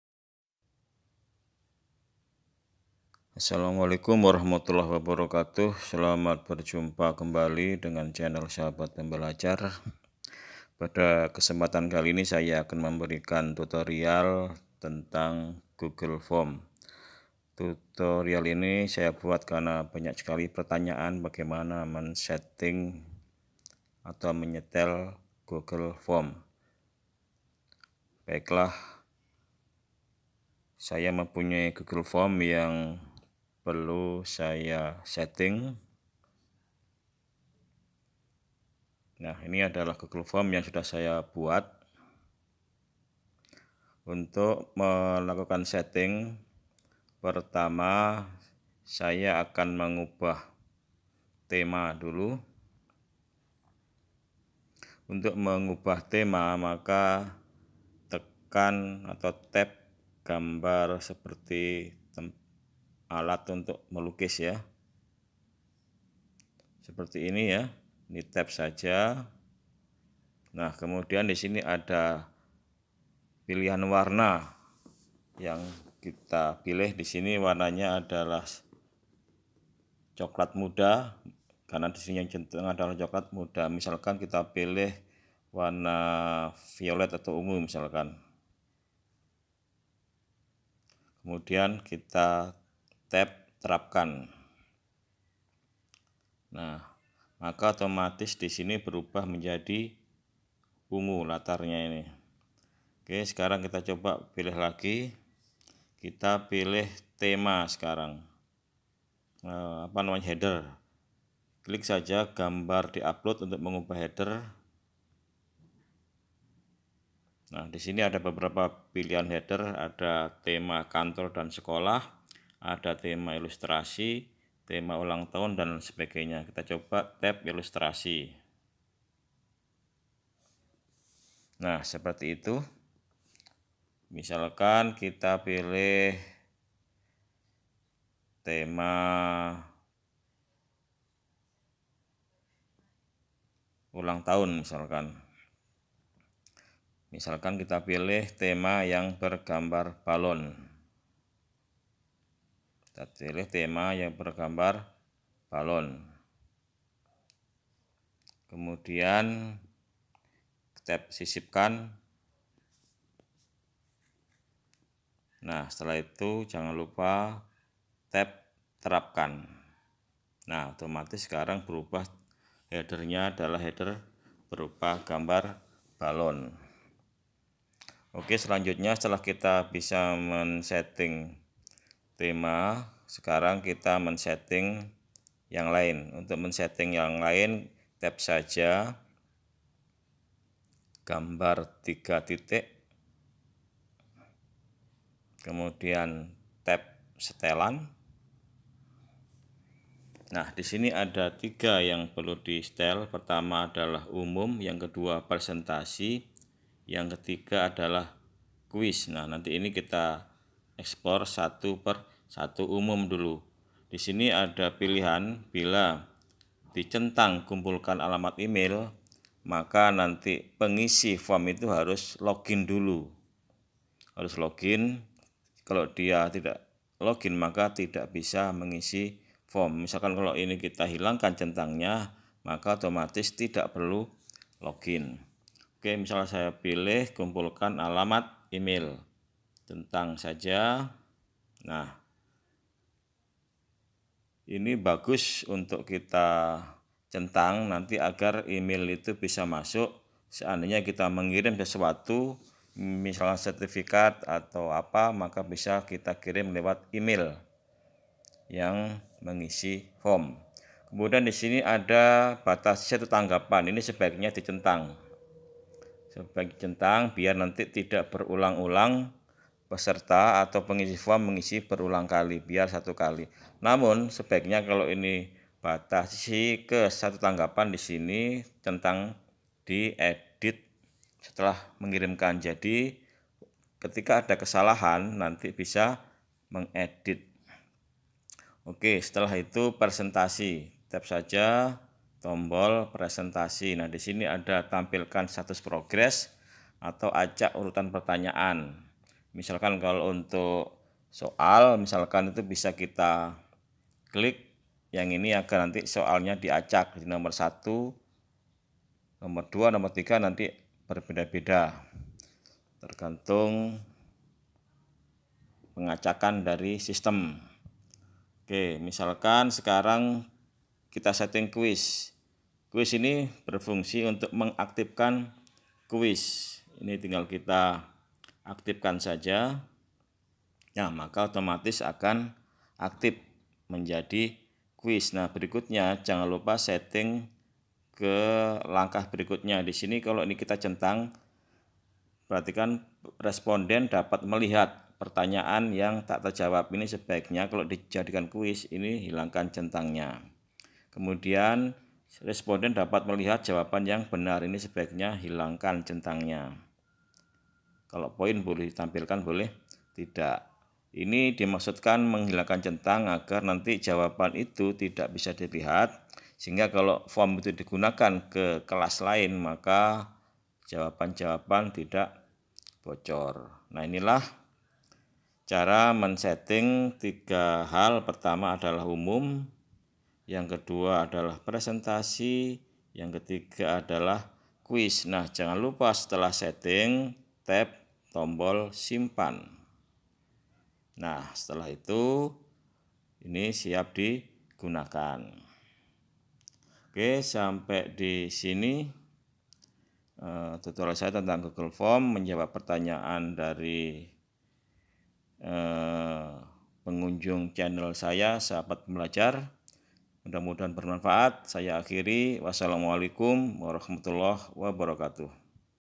SETTING GOOGLE FORM UNTUK SATU KALI TANGGAPAN
Tutorial google form, setting dasar yaitu permintaan email, berapa kali tanggapan, setting kuis dll. Ini semua dapat dilakukan dengan HP.

Form sebaiknya disetting satu kali tanggapan biar tidak ada orang yang ngisi berkali-kali. Begitu juga kuis, sebaiknya disetting untuk satu kali tanggapan